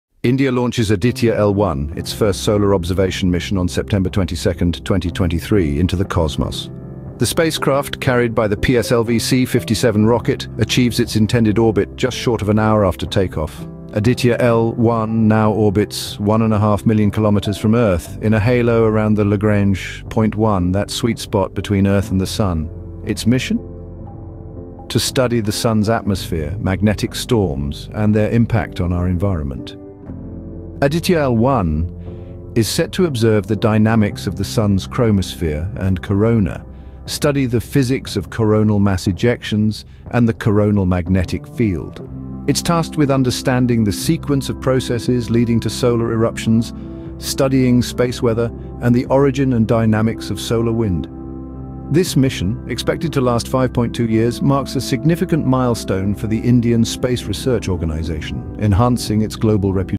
Solar Mission Aditya L1| India's First Leap to the Sun
Halo-Orbit Insertion of Solar MissionAditya-L1had  been Successfully Accomplished.

The orbit of Aditya-L1 spacecraft is a periodic Halo orbit which is located roughly 1.5 million km from earth on the continuously moving Sun – Earth line with an orbital period of about 177.86 earth days. This Halo orbit is a periodic, three-dimensional orbit at L1 involving Sun, Earth and a spacecraft. This specific halo orbit is selected to ensure a mission lifetime of 5 years, minimising station-keeping manoeuvres and thus fuel consumption and ensuring a continuous, unobstructed view of sun.